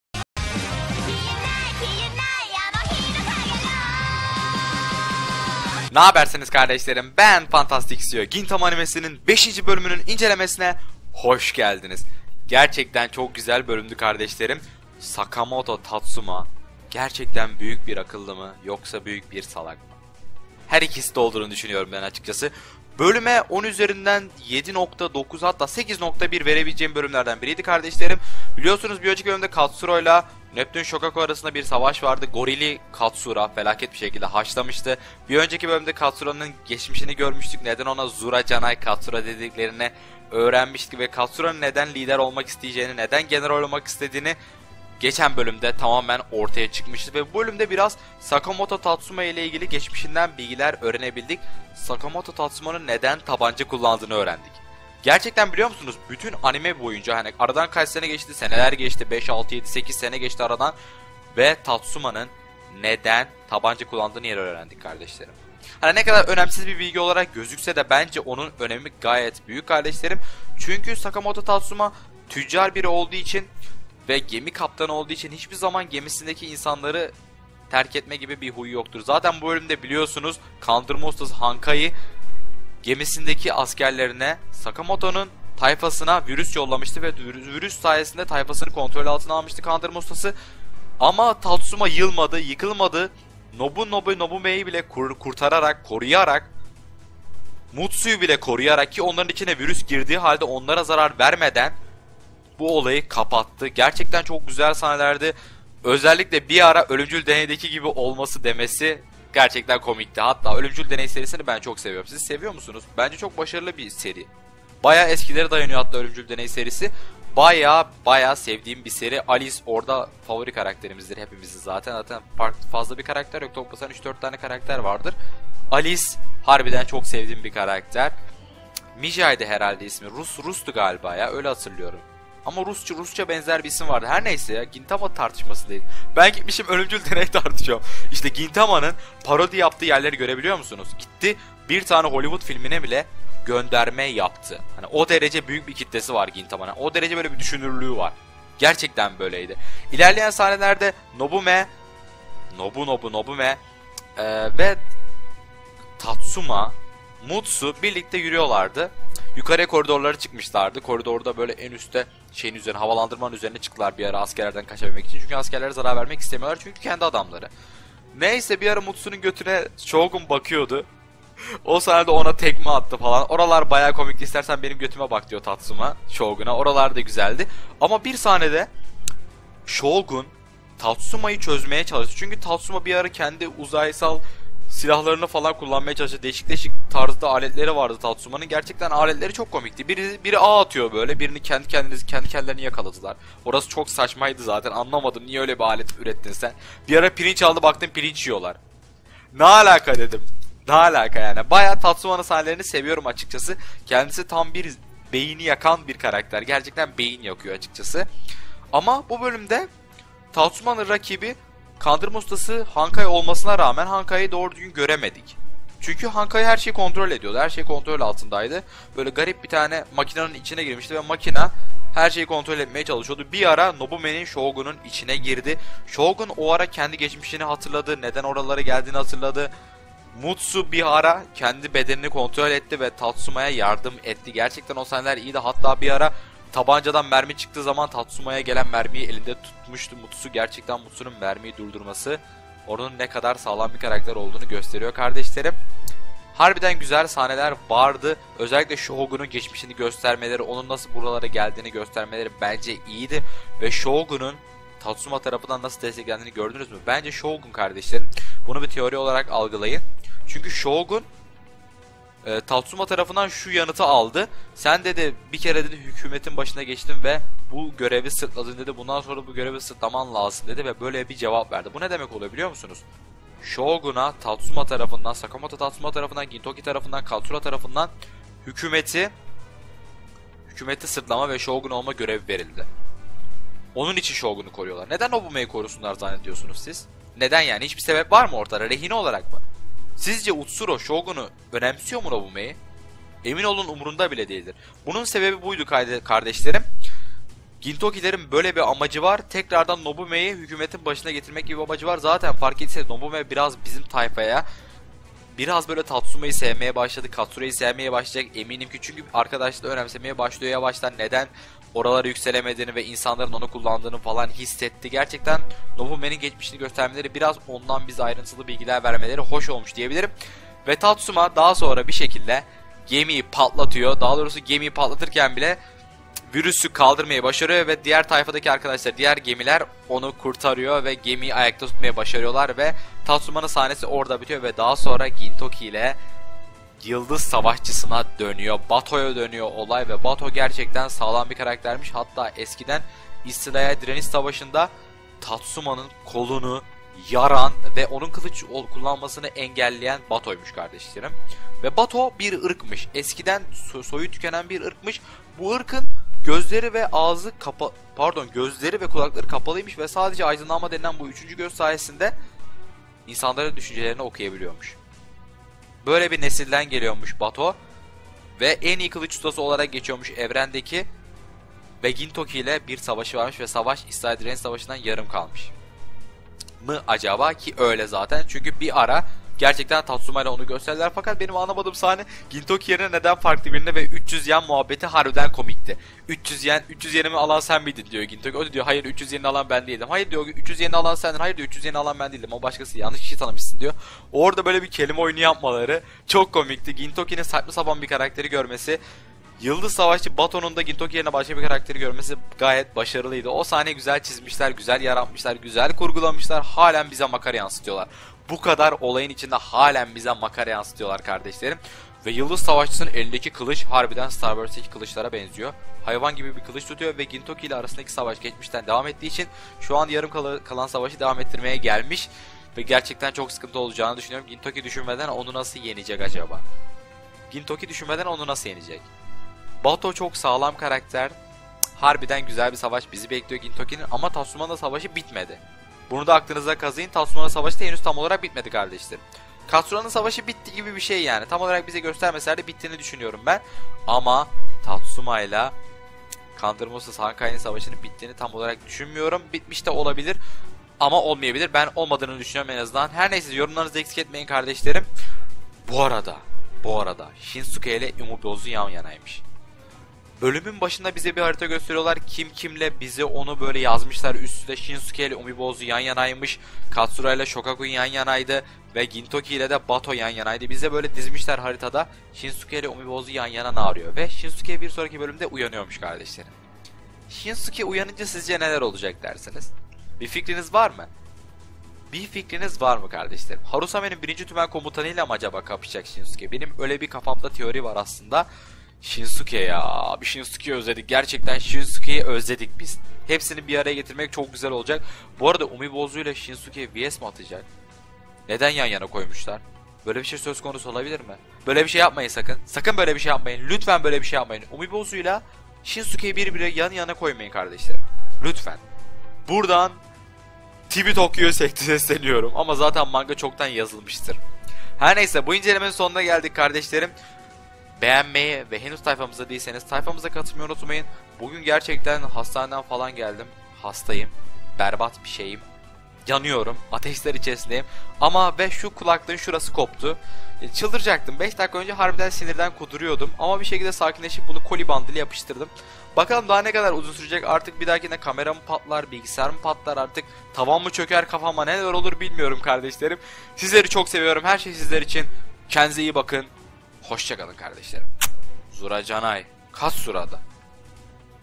Gintama (2017) 5.Bölüm İncelemesi l 銀魂
Nabersiniz kardeşlerim,Sakamato Tatsuma,Kandırma Ustası Hankai'ye harika bir darbe vurdu.Shogun Nobunobu'nun geçmişinden kareler gördük.Gintoki ve ekibi sizce Nobunobu'yu tekrardan Shogun olmasını ister mi dersiniz?

İzlediğin için çok teşekkürler kardeş!

FANTASTİK KARDEŞ YUMRUĞU - FANTASTİK TAYFA!